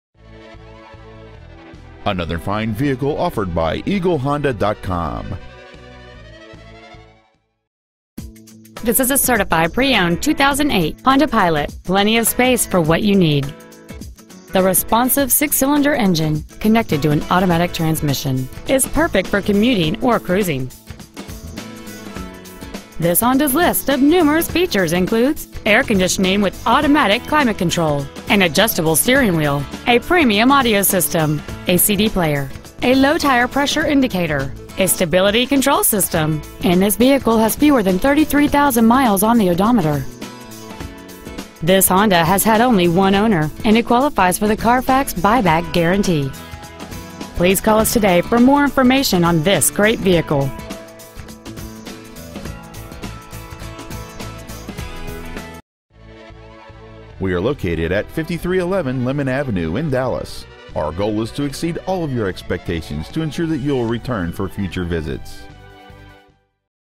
2008 Honda Pilot Irving TX
Year: 2008
Make: Honda
Model: Pilot
Engine: 3.5L SOHC MPFI 24-valve i-VTEC V6 engine
Trans.: Automatic
Exterior: Red
Miles: 32,671

John Eagle Honda Dallas
5311 Lemmon Ave